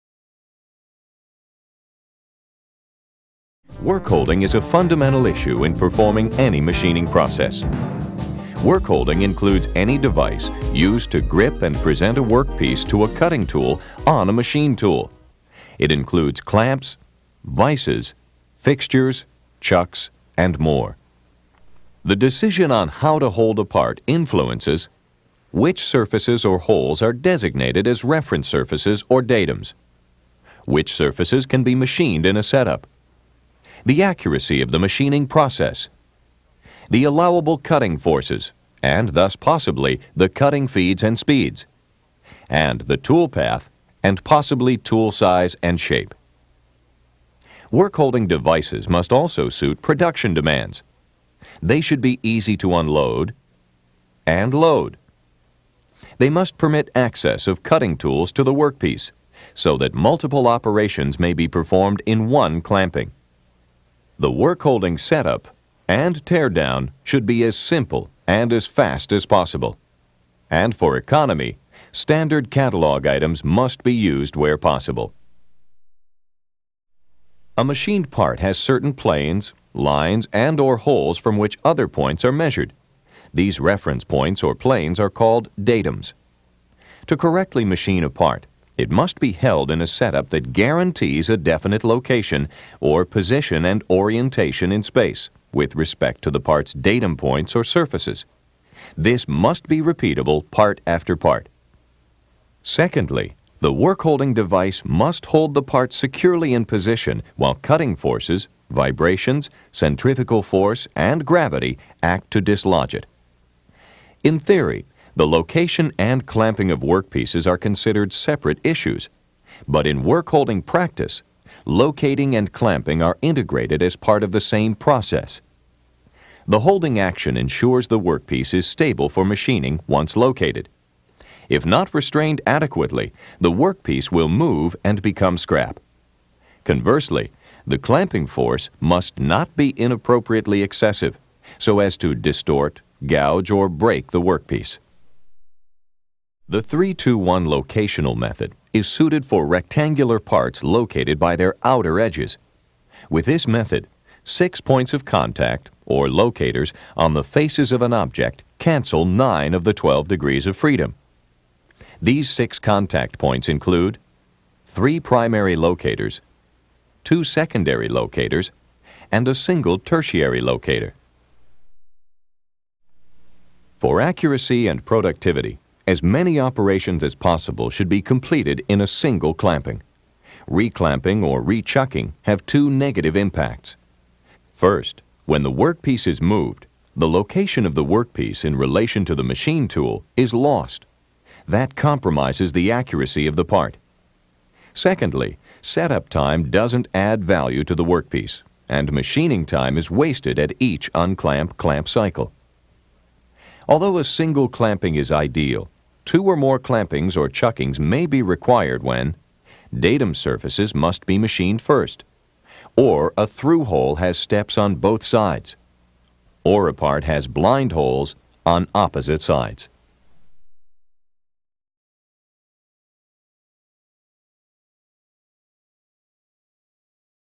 Work holding
Covered Points:
- Holding methods.
- Holding setup.
- Holding factors.
- Charactrstic of holding.